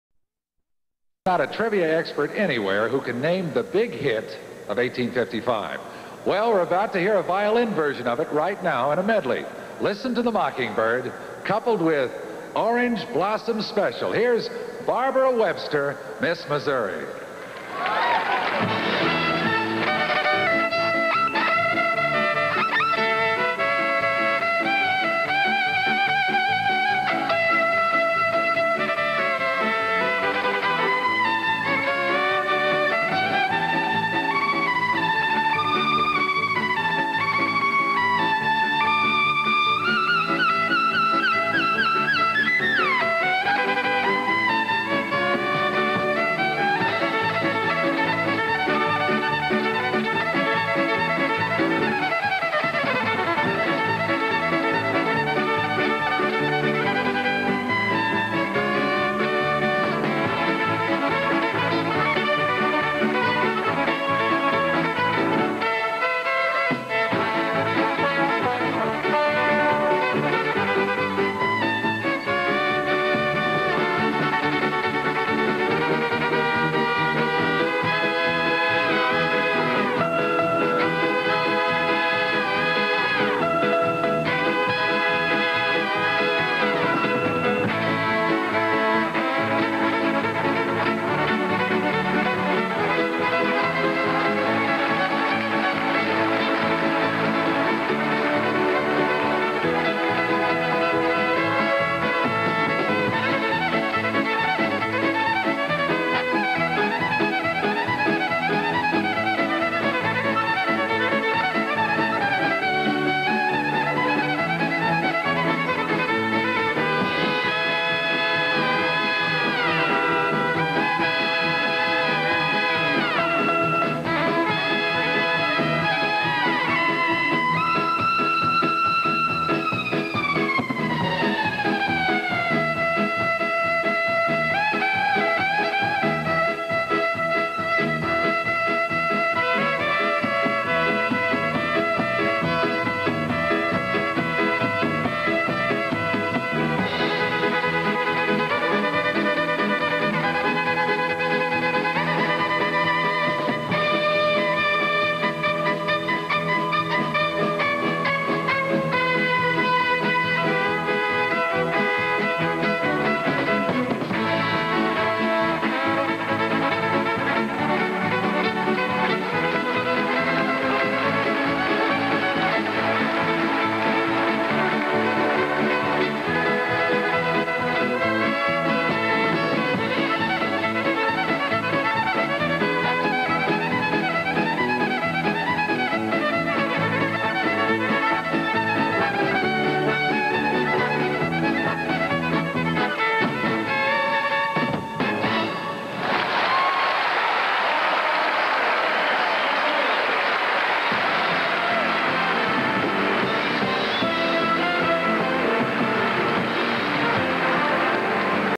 Barbara Webster Talent
Barbara Webster performs her talent in the Miss America Pageant held in Sept., 1983